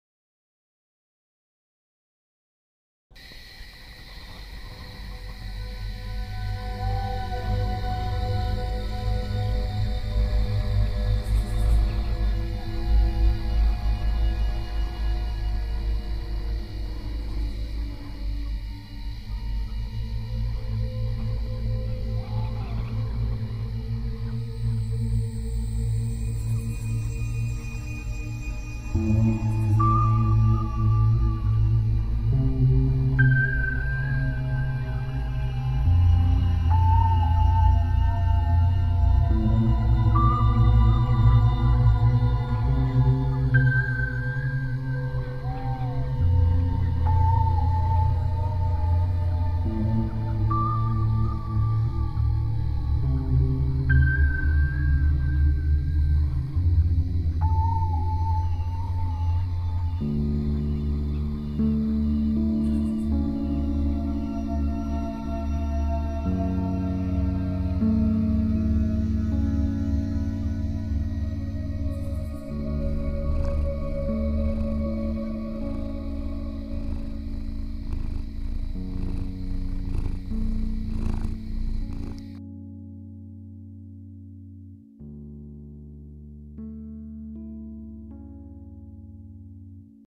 Mood Change Animation with Sound
Created in Art 197A 3D Animation Fundamentals, Santa Ana College 3D Modeling and Animation Certificate Program 2013.